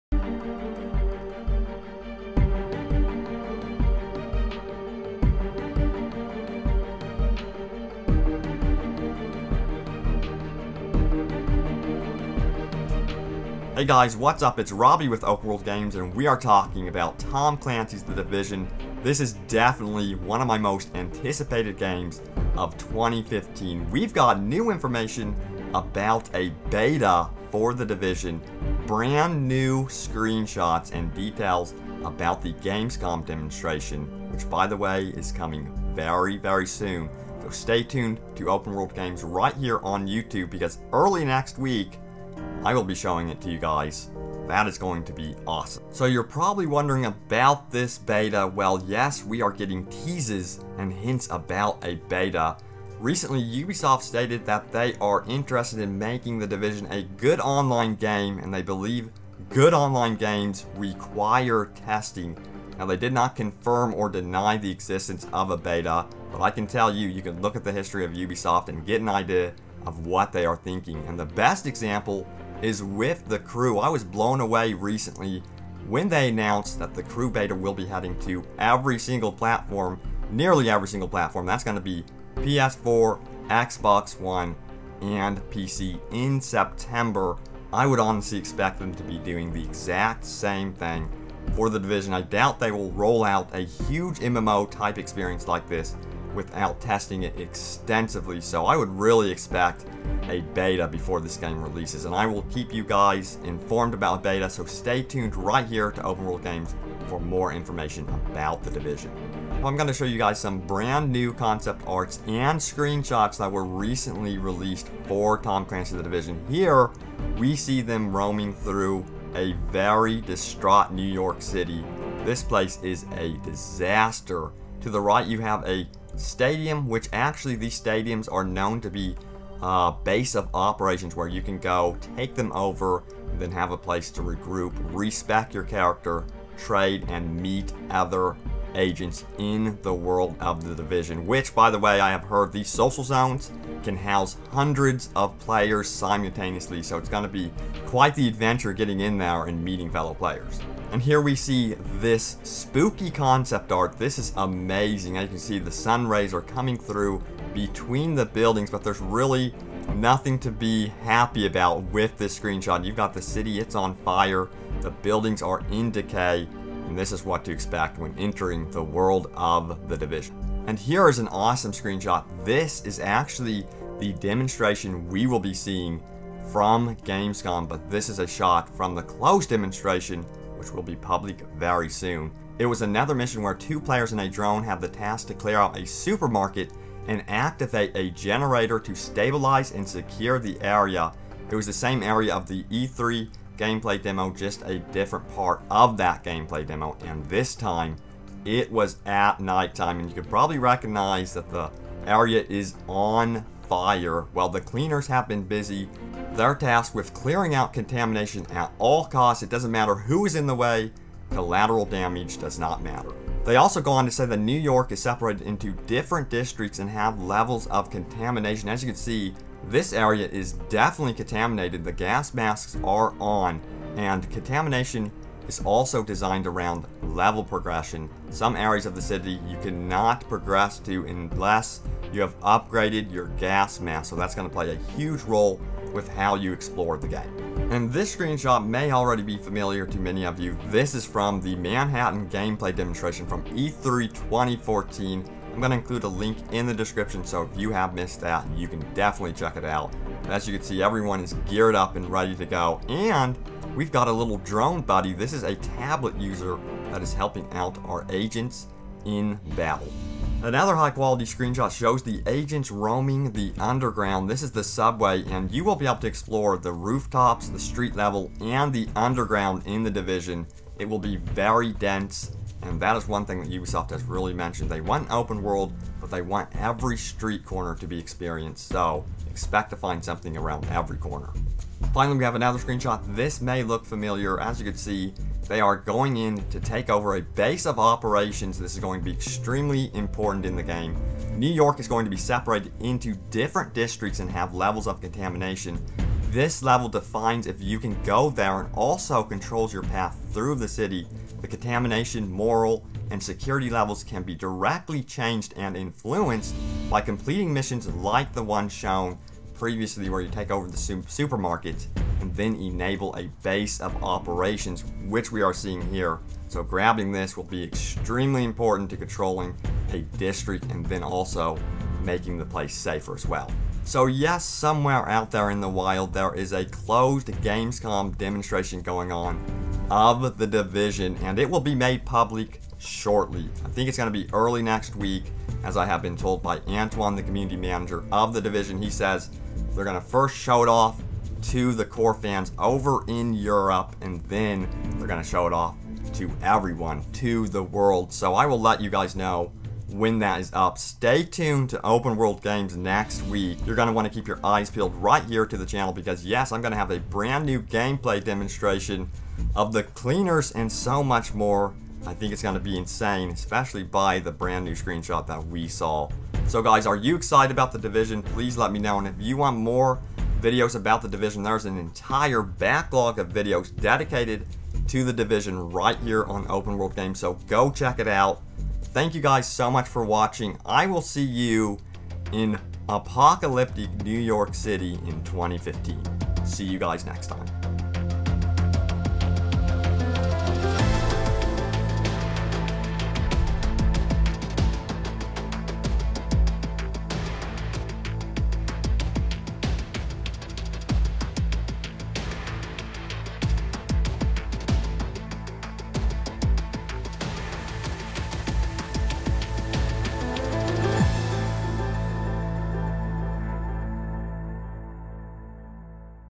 The Division Beta Teased! New Gameplay Demo From GamesCom Soon! New 4k Screens!
NEW! Tom Clancy's The Division beta teasted, The Division new gameplay gamescom 2014 demo soon, new Division screenshots on PS4, Xbox One, PC.

UPDATE: Special thanks to MarcoStyleNL for the info Proper shoutout coming soon!  Info in video originally from the forums.